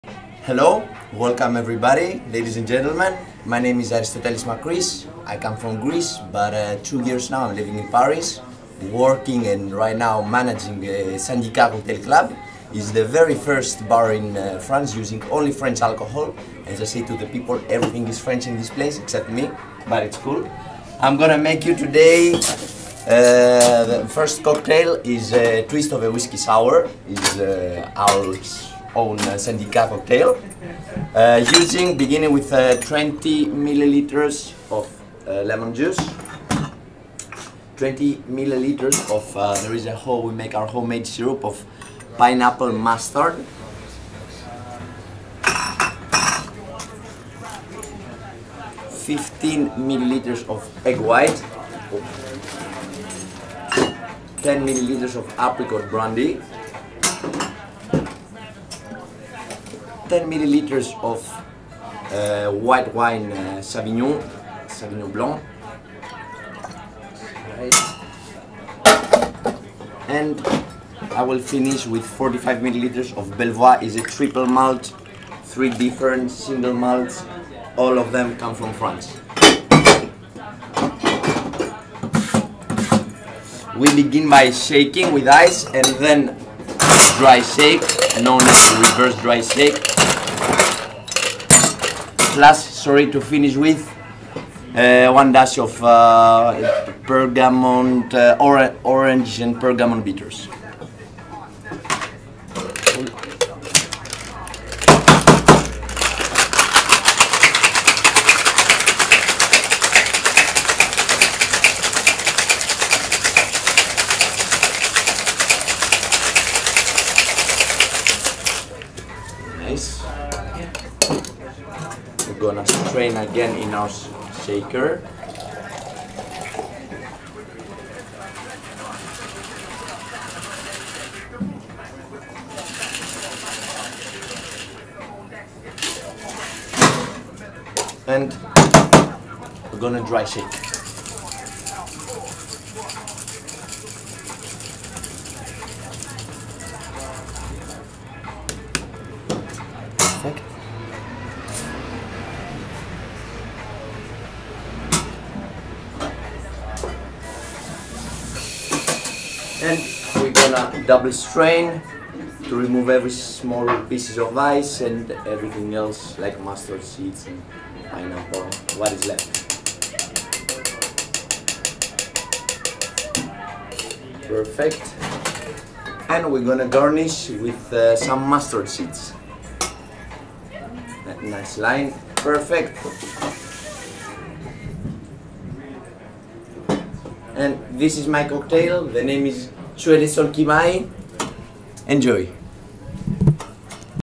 Aris Makris from Le Syndicat (Paris) : ​ Tu es le seul qui maille
Aris Makris from le Syndicat makes for us a twist on a whisky sour: Tu es le seul qui maille